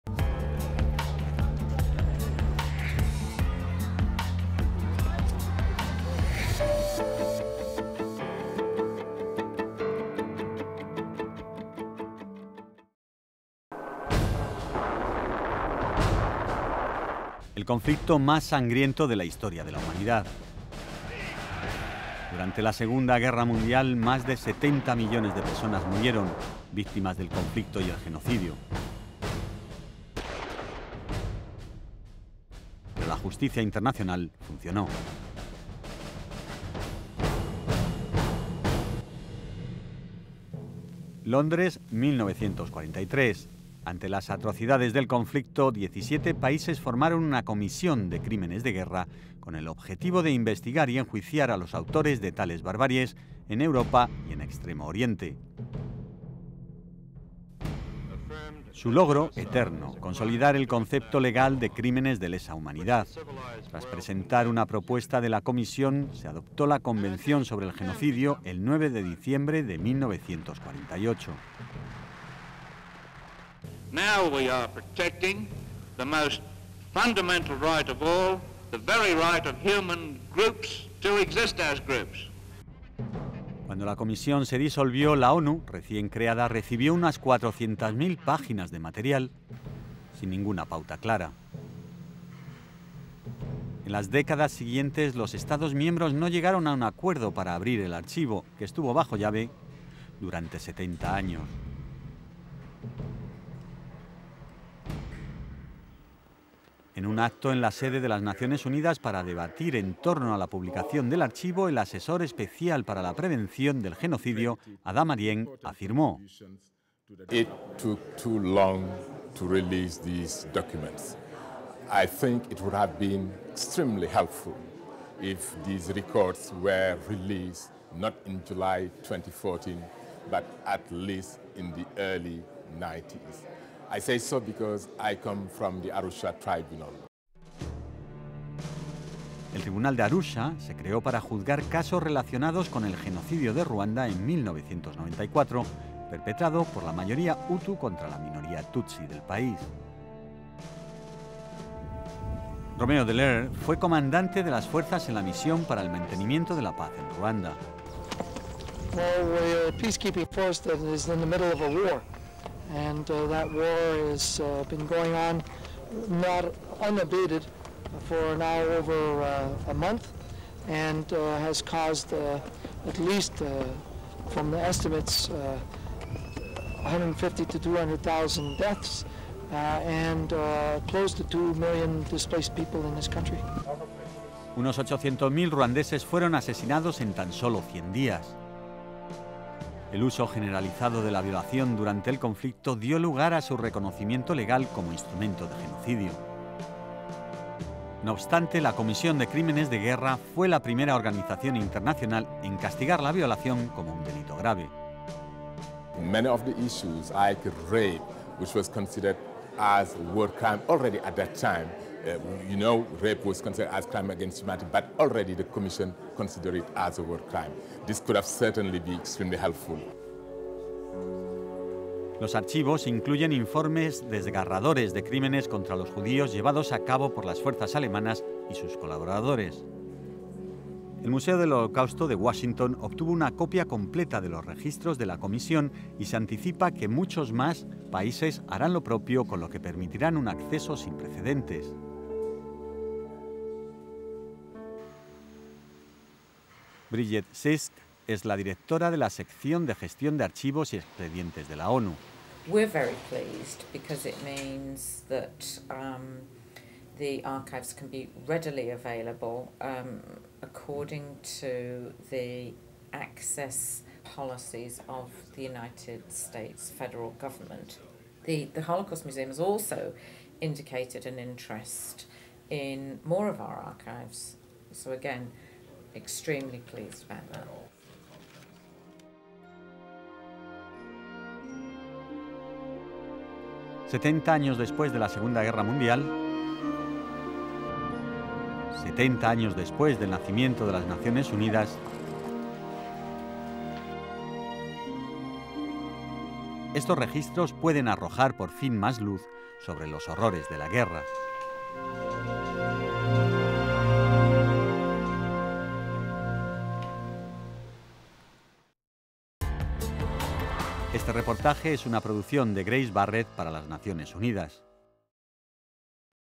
El archivo olvidado de la II Guerra Mundial
Durante 70 años, cuatrocientas mil páginas conteniendo las investigaciones y conclusiones de la Comisión de Crímenes de Guerra, creada tras la II Guerra Mundial, han permanecido bajo llave. Ahora, las Naciones Unidas estudian cómo publicar ese archivo para el conocimiento general.
El Día Internacional para la Conmemoración y Dignificación de las Víctimas del Crimen de Genocidio y para la Prevención de ese Crimen se conmemora anualmente el 9 de diciembre
Si deseas más información sobre este importante asunto, visita nuestras páginas